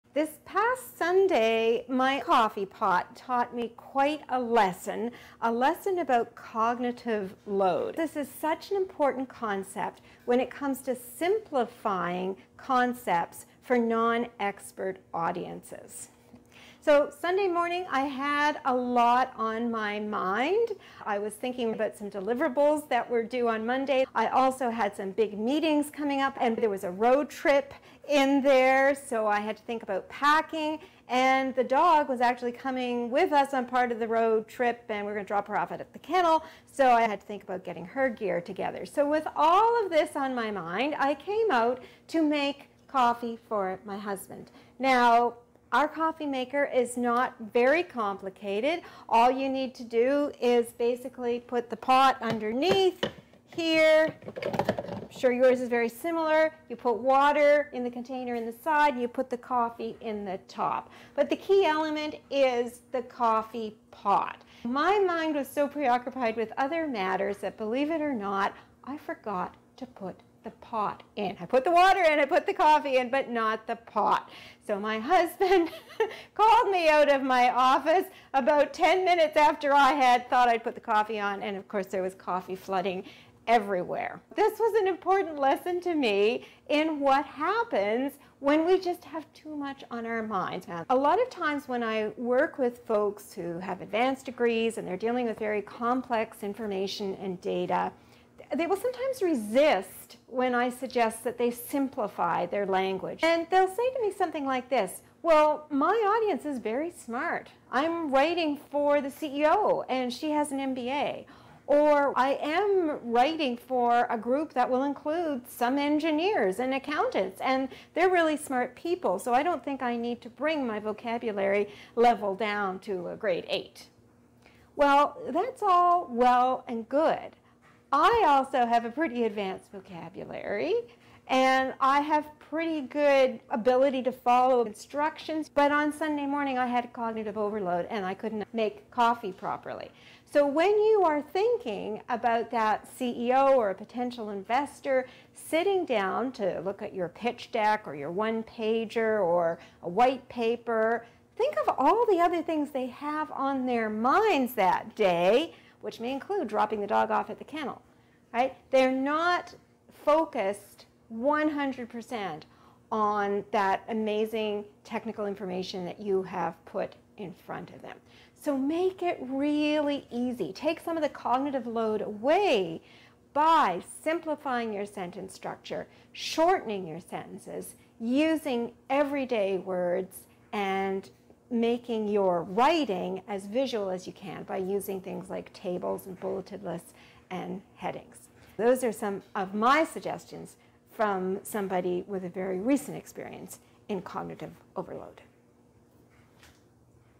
Fable of the coffee pot
A friend of mine, who also happens to be a wise entrepreneur, makes it a practice to end each day by asking herself how she failed and what she learned from the mistake.

Here’s what I learned from a catastrophic coffee-making failure last weekend. May it save you from committing a common mistake I see a lot of researchers and researcher-entrepreneurs making: overburdening a lay audience with so much detail that the result is cognitive overload.